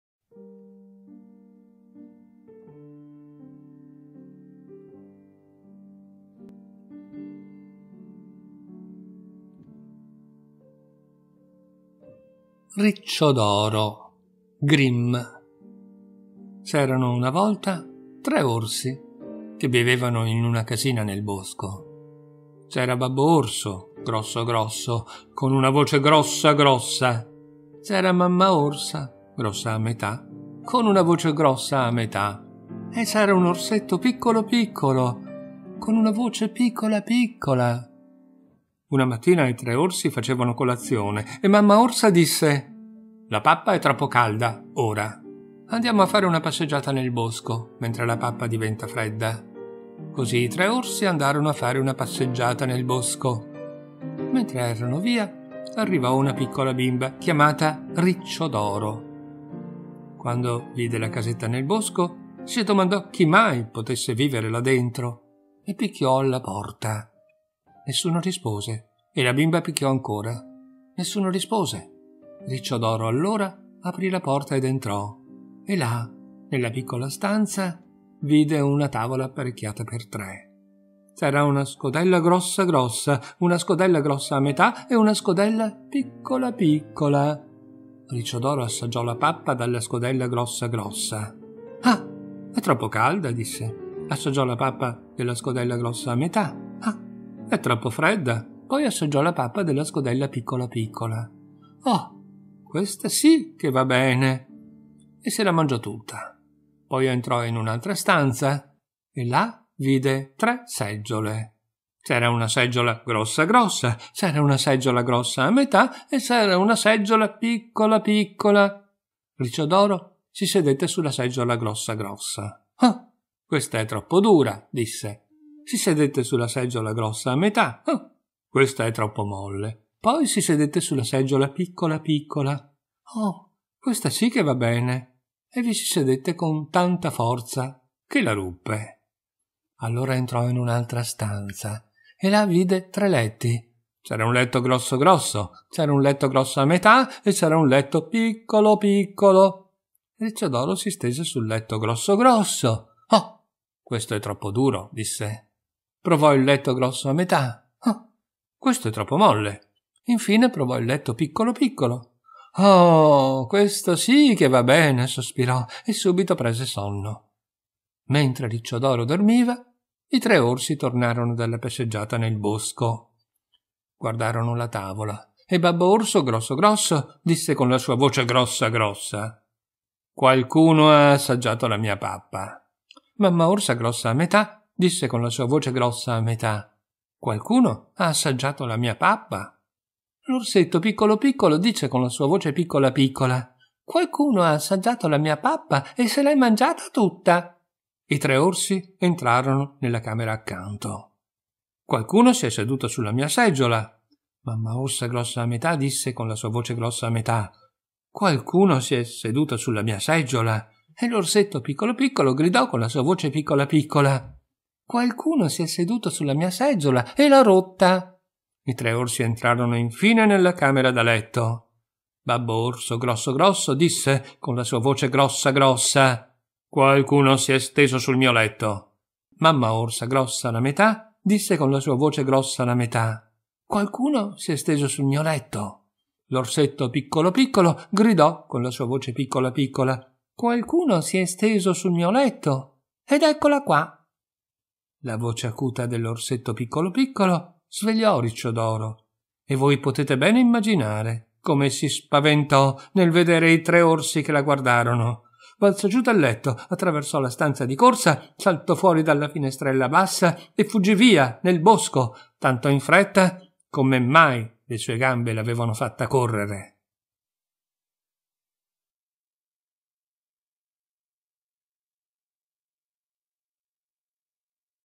RICCIODORO (GRIMM) - fiaba
una fiaba al giorno...
racconta valter zanardi